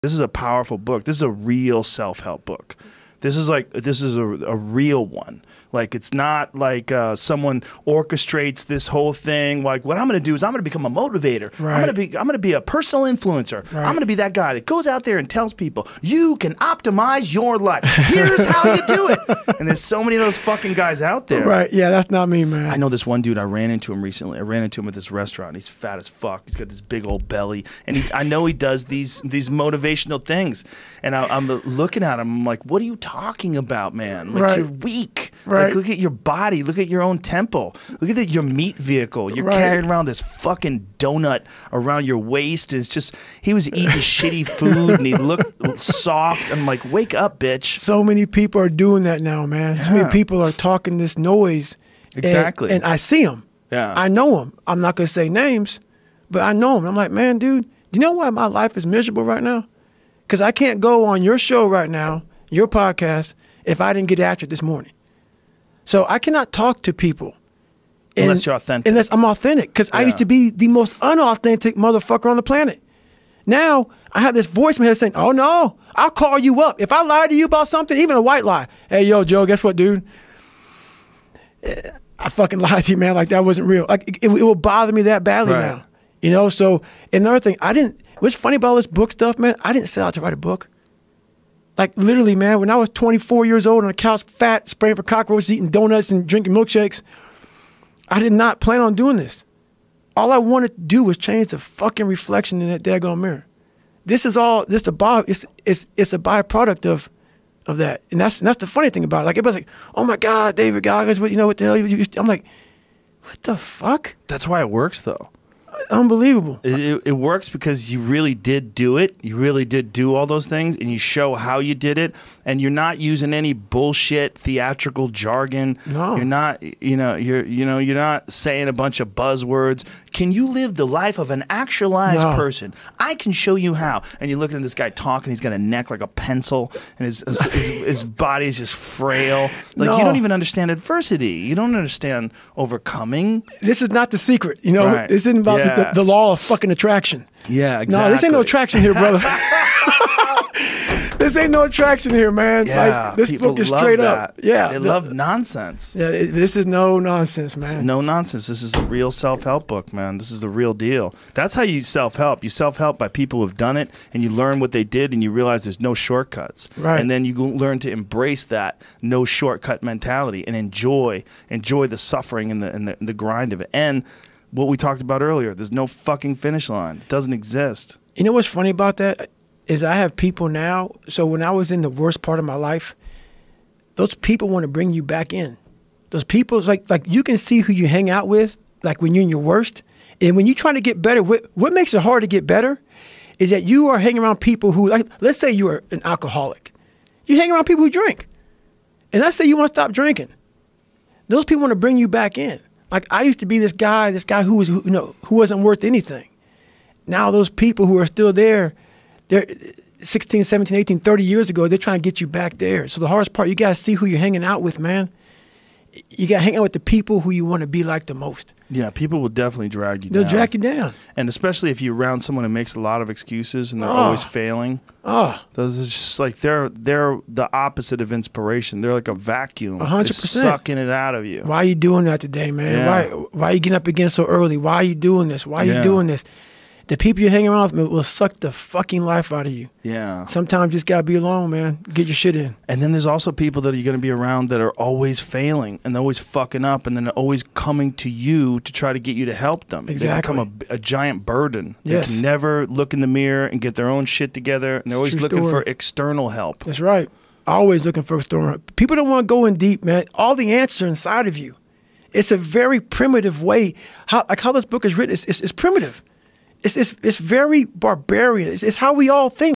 Cut Negative People out of Your Life to Become Successful | David Goggins
Joe Rogan and David Goggins talk about the type of people who will hinder your success and make it harder for you to grow.